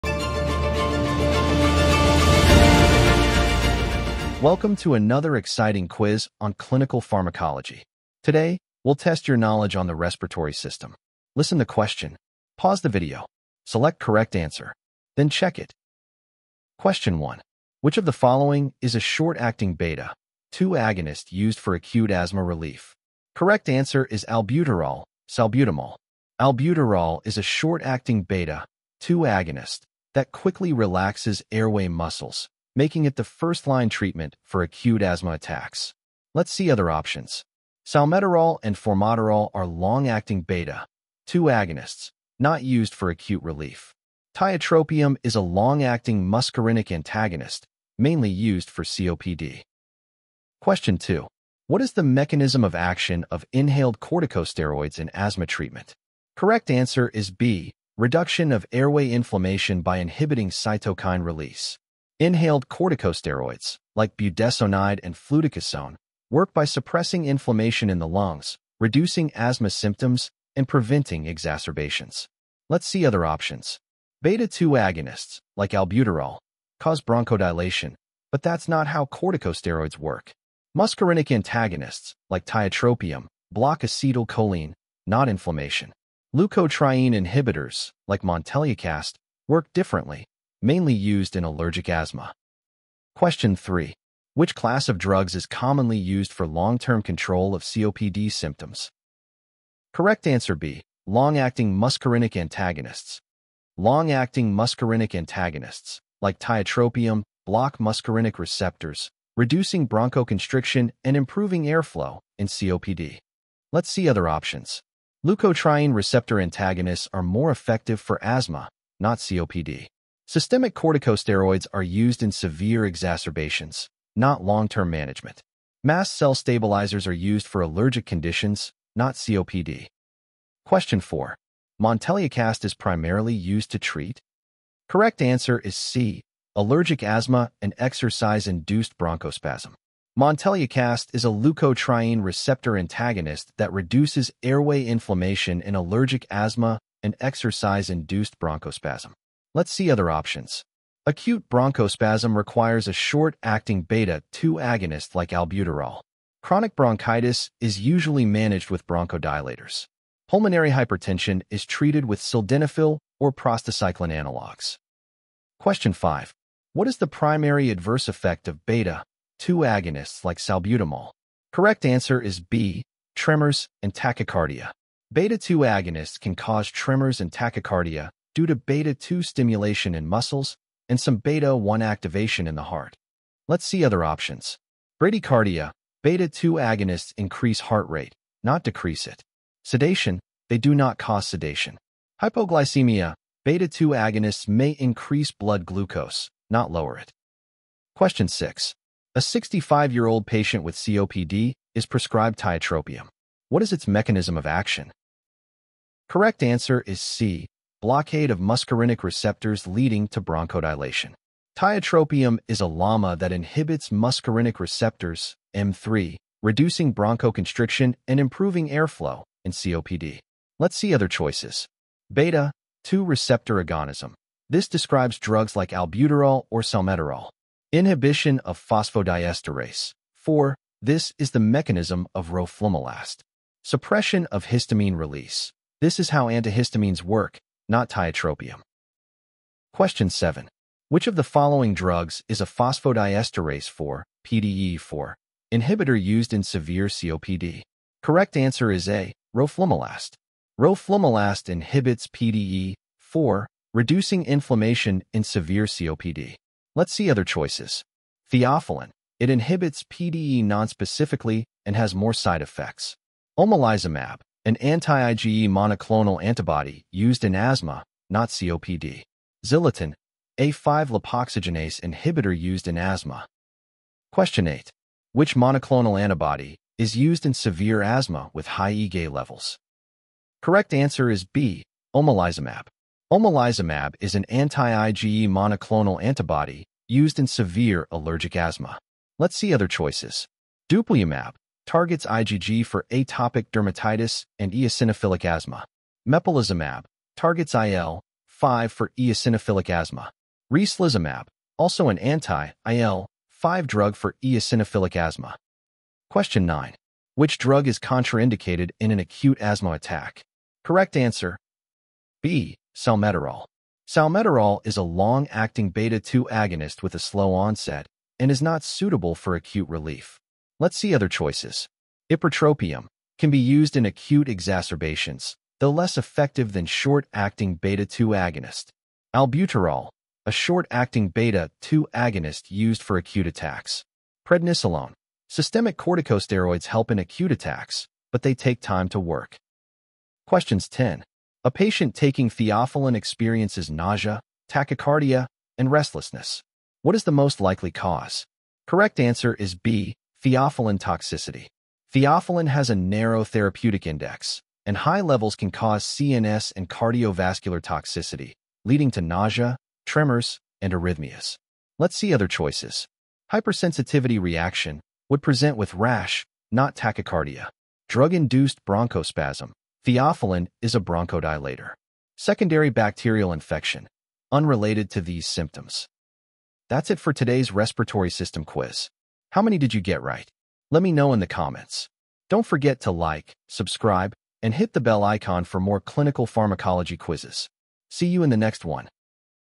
Clinical Pharmacology Quiz! Test Your Knowledge
Think you know asthma, COPD, and respiratory meds inside out? Let’s find out! This video covers 10 clinically relevant MCQs with clear explanations – perfect for pharmacy, medical, and nursing students!

📌 What You’ll Learn:
✔️ Short-acting vs long-acting bronchodilators
✔️ Inhaled corticosteroids mechanism
✔️ COPD drug classes
✔️ Common side effects of respiratory drugs
✔️ Clinical use of leukotriene antagonists

🎓 Stay sharp. Stay curious. Stay Drug Smart.